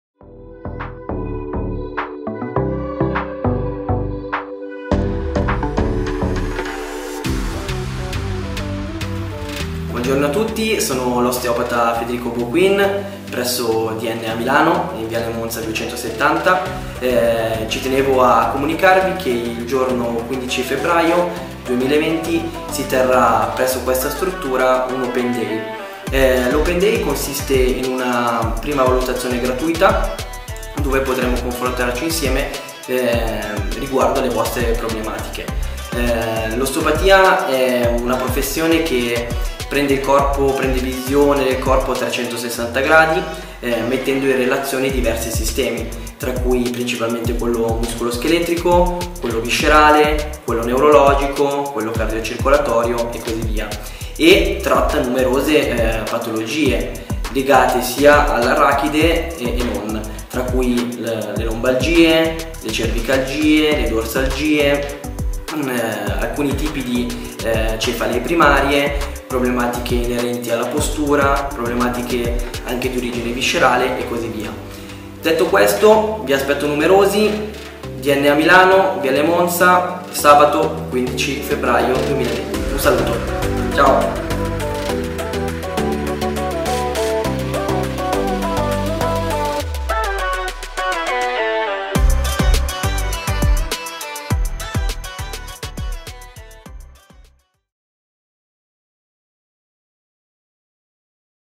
15 FEBBRAIO 2020 - Osteopatia | Open day! VALUTAZIONE OSTEOPATICA GRATUITA!
GIORNATA APERTA dalle 9.00 alle 13.00
Valutazione gratuita dello STATO DI SALUTE DI OSSA, MUSCOLATURA E/O EVENTUALI DISTURBI con il Dott. Federico Bouquin.